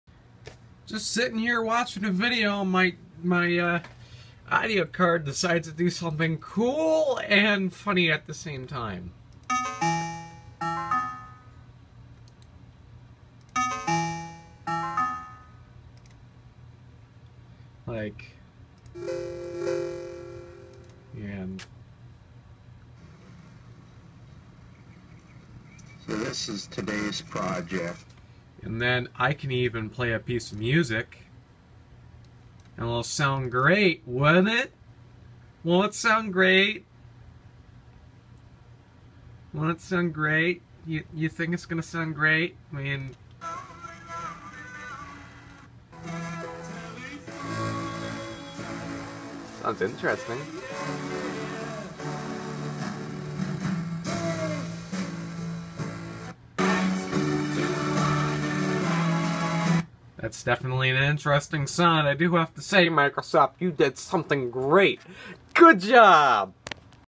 Sound card goes 16bit!
Just watching a video and sound card decides "hey want me to sound like your on a commadore 64?"